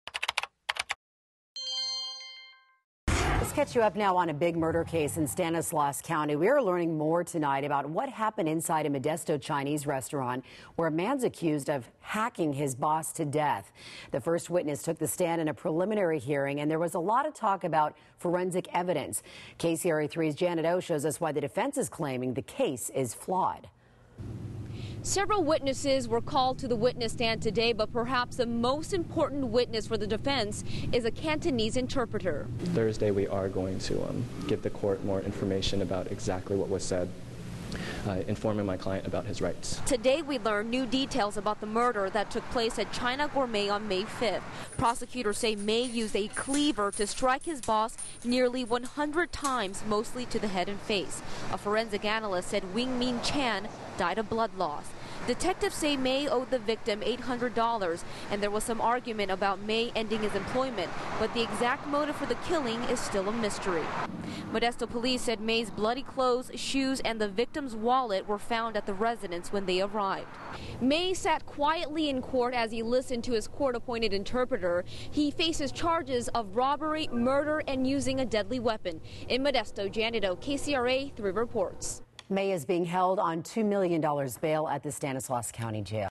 Preliminary Hearing For Meat Cleaver Murder
A preliminary hearing began for the man accused of killing his boss with a meat cleaver inside a Modesto chinese resturant.